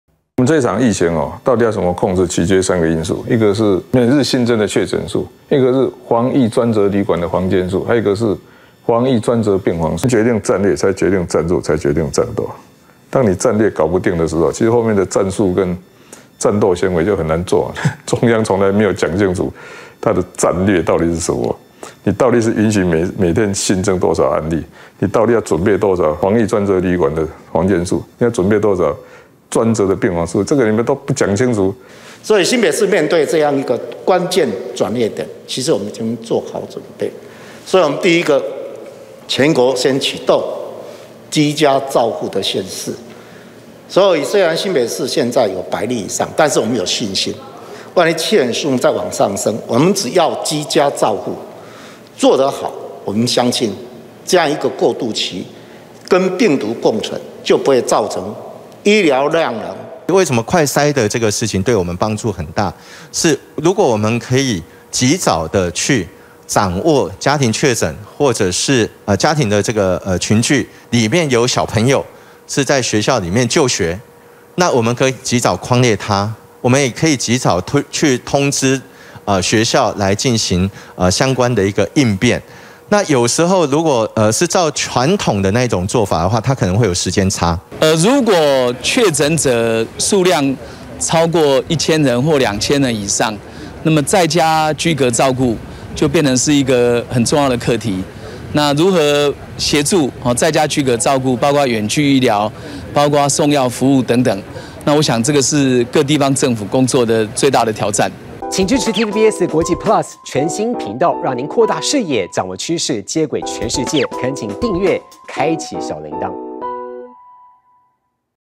「北北基桃」防疫挑戰 柯文哲轟:戰略不明｜TVBS新聞
今天本土確診暴增744例，創下有疫情以來的新高紀錄，未來確診數恐怕只會不斷攀高，TVBS舉辦"從清零到共存"防疫論壇，我們也訪問到各縣市首長，對於現在台灣社區本土確診不斷暴增，在現行的防疫政策上，他們遇上什麼挑戰和隱憂，首先來聽聽被指揮官陳時中點名疫情最嚴重的熱區，也就是"北北基桃"的首長們的心聲。